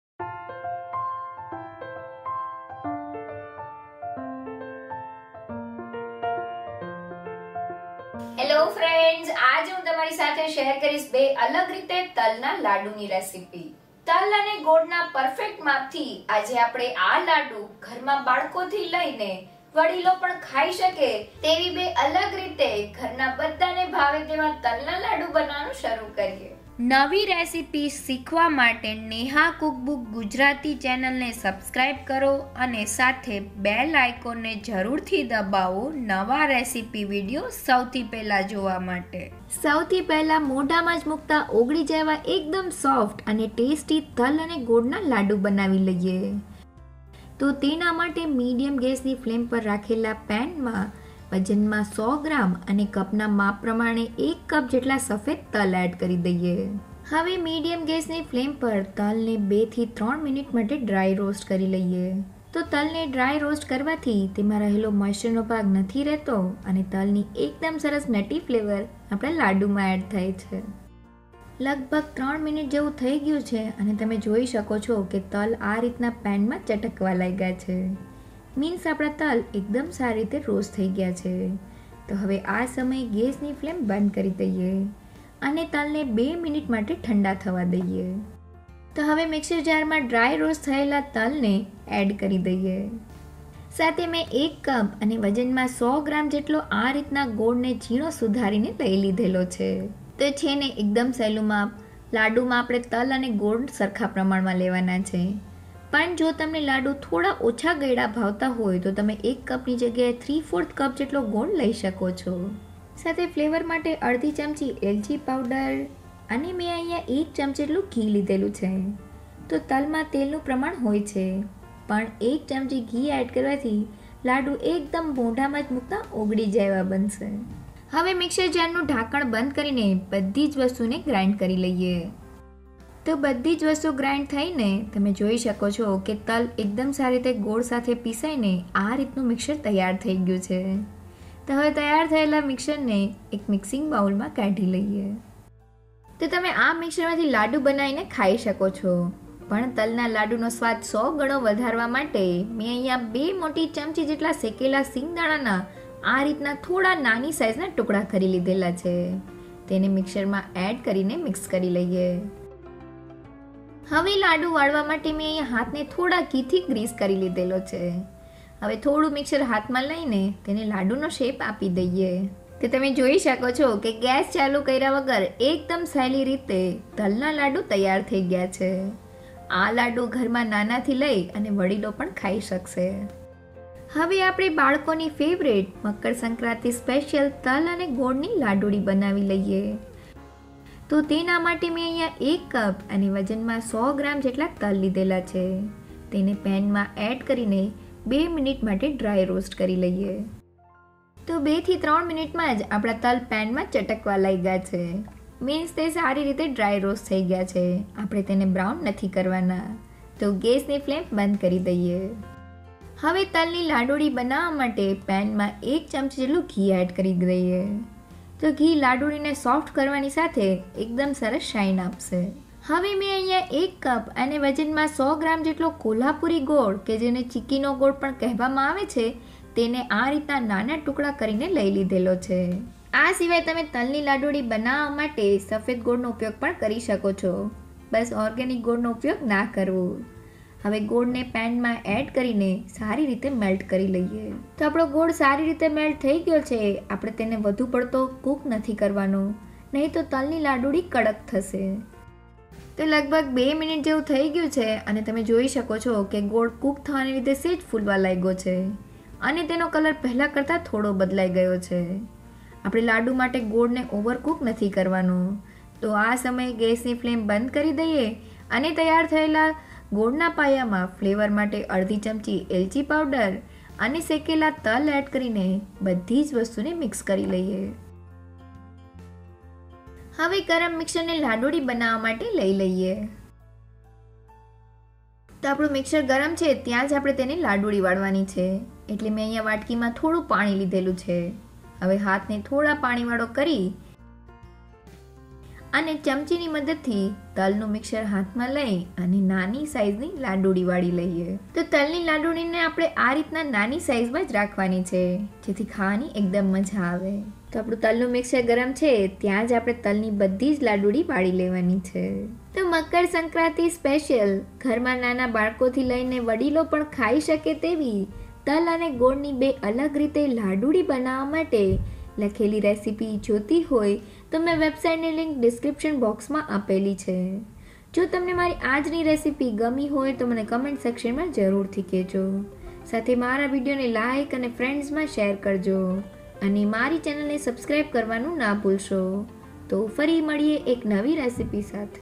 2 અલગ રીતે બધાને ભાવે તેવા તલ ગોળના લાડુ | Til gud ladoo recipe | Tilkut recipe
ઢોકળા રેસીપી / dhokla recipe